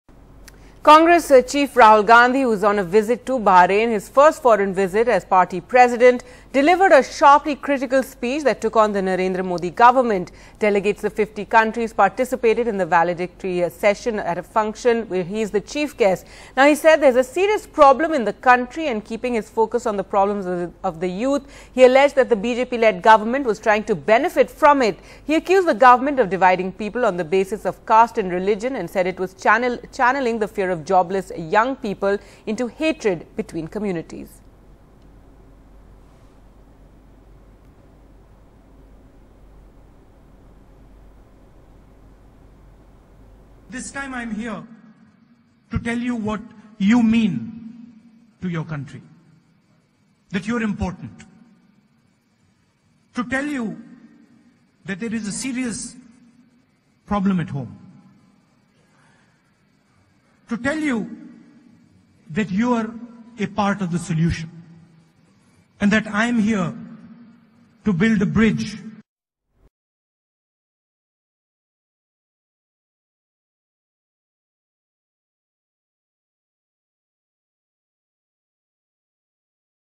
"Serious Problem At Home": Rahul Gandhi Tells Indians In Bahrain
Congress chief Rahul Gandhi, who is on a visit to Bahrain, on Monday made his case before the Indians residing there, delivering another sharply critical speech that took on the Narendra Modi government. Delegates of 50 countries participated in the valedictory session of a function, where the 47-year-old, who recently took over the reins of India's largest party, is the chief guest. "I am here to tell you what you mean to our country, that you're important, to tell you there is a serious problem at home, to tell you that you're part of the solution and that I am here to build a bridge between wherever you are in the world and home," he said in his address to the Global Organisation of People of India Origin.